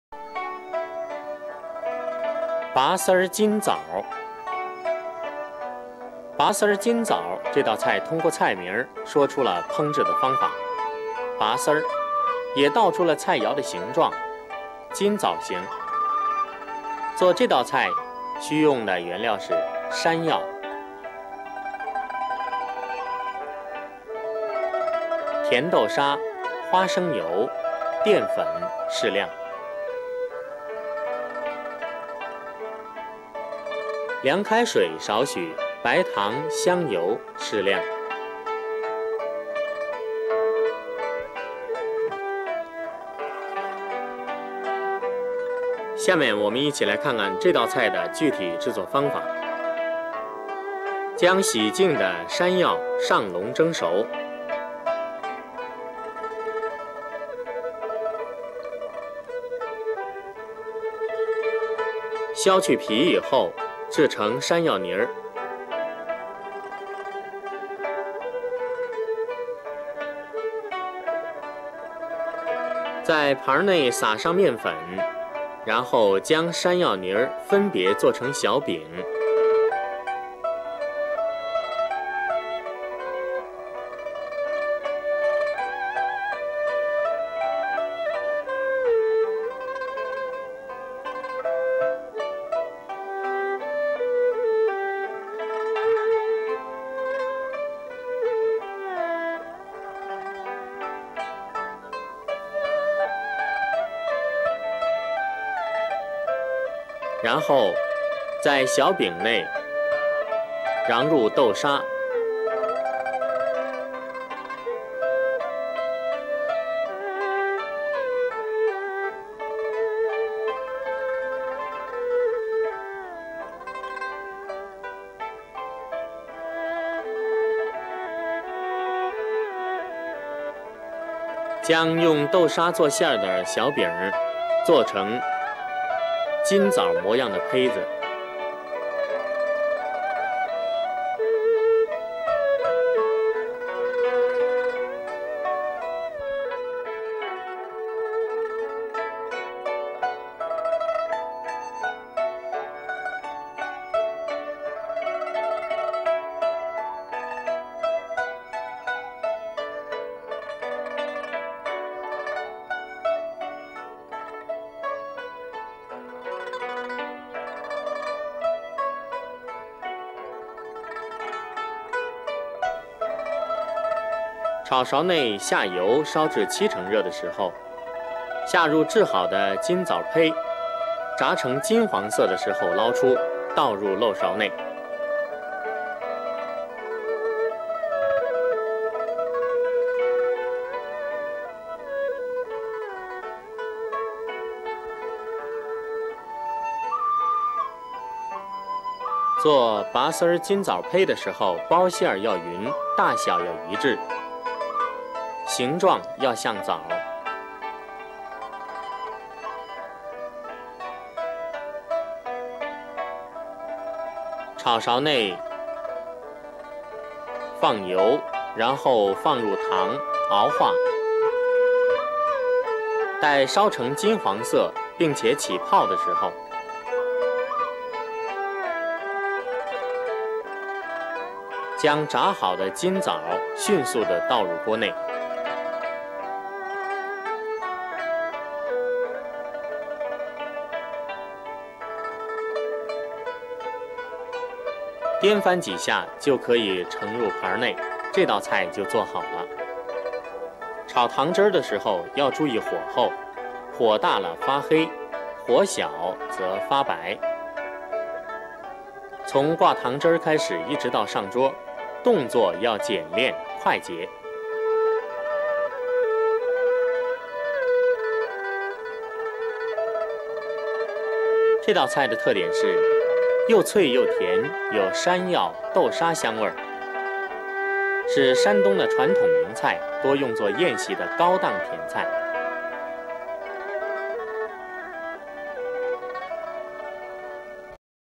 中国美食， 中国美食， 八大菜系 徽菜经典名菜之 拔丝金枣
Eight Regional Cuisines in China/these teaching videos are from 30 years ago, they are very classic and cold be used for Chinese cooking learning